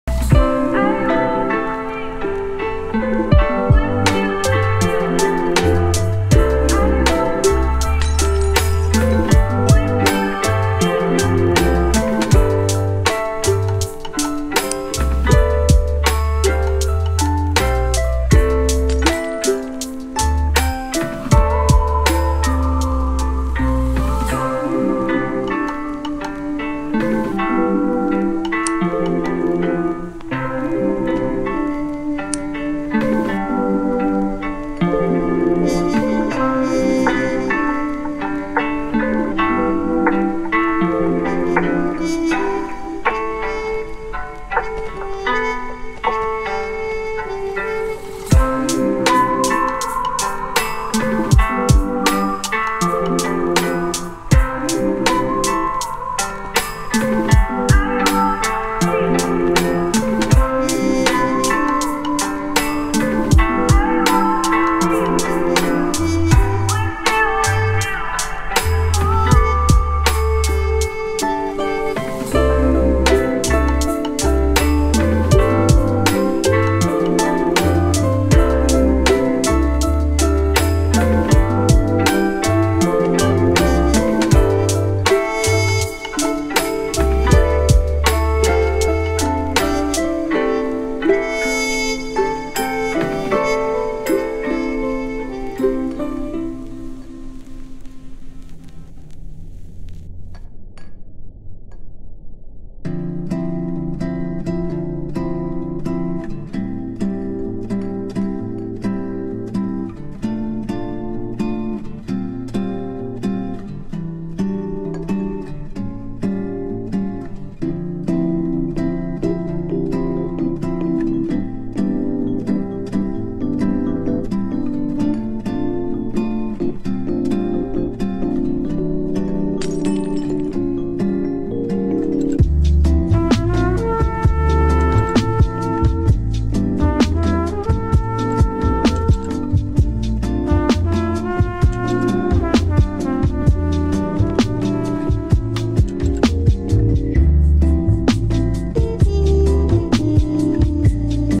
Calm tea party ( read description)
After the events  the guy that broke the table remembered how to fix a table and had a tea party￼ :]